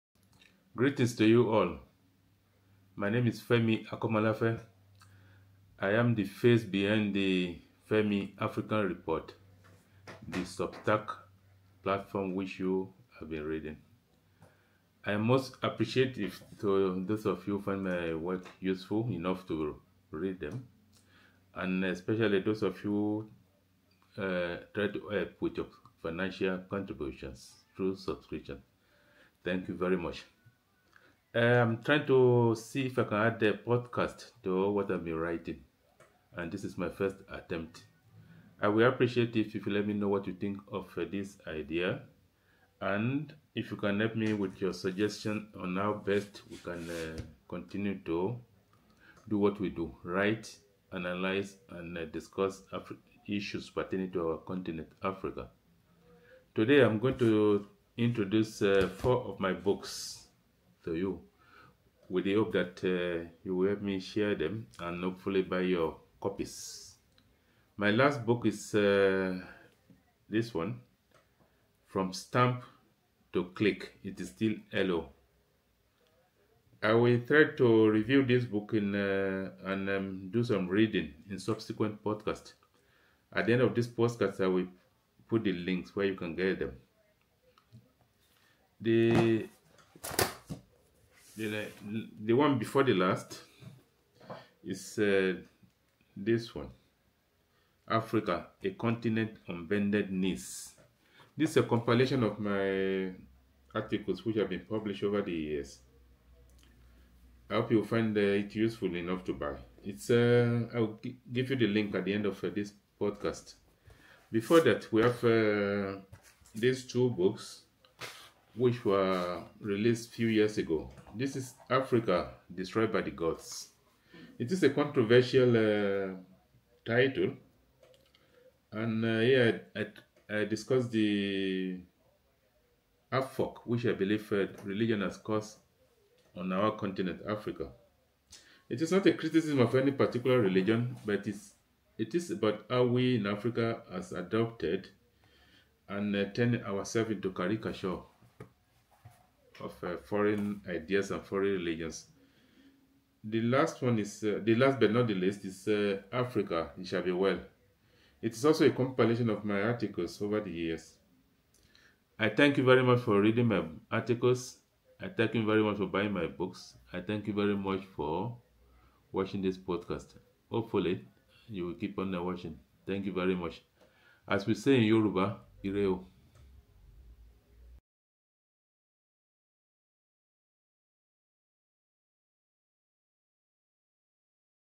Introducing Femi African Report
This short video was made to introduce My Substack platform which I use for my Pan-African writings.

And, please help me to share it.